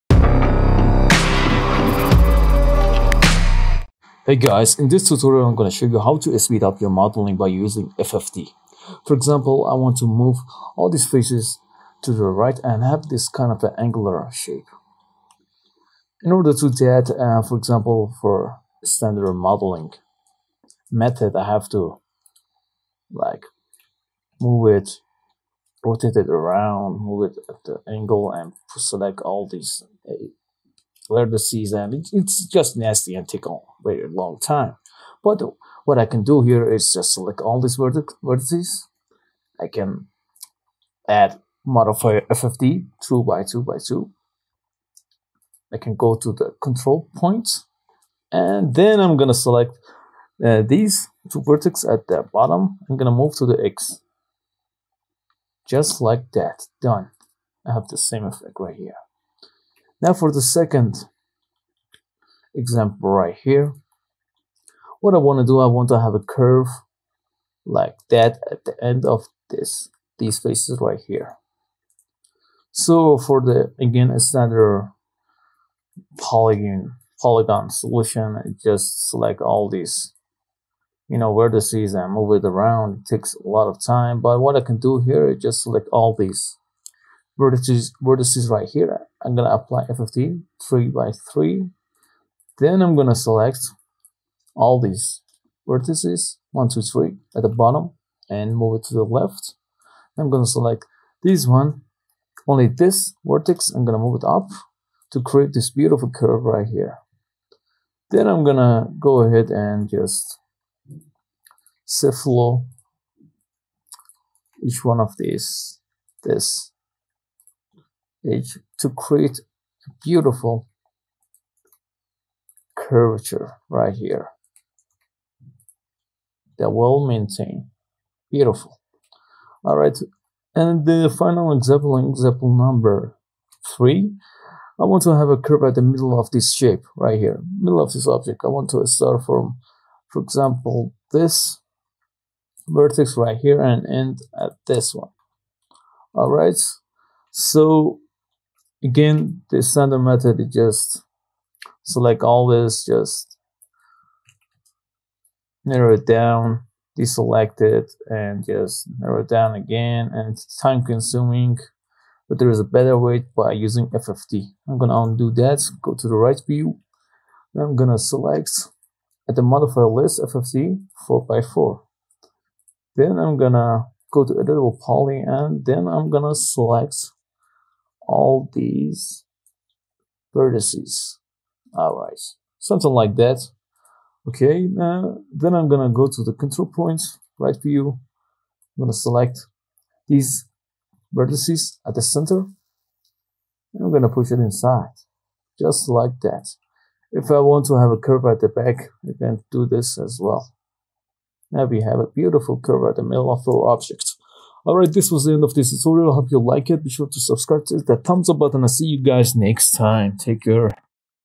Speed Up 3d Modeling With FFD modifier
In this tutorial, I show you how to use FFD to your advantage for 3d modeling.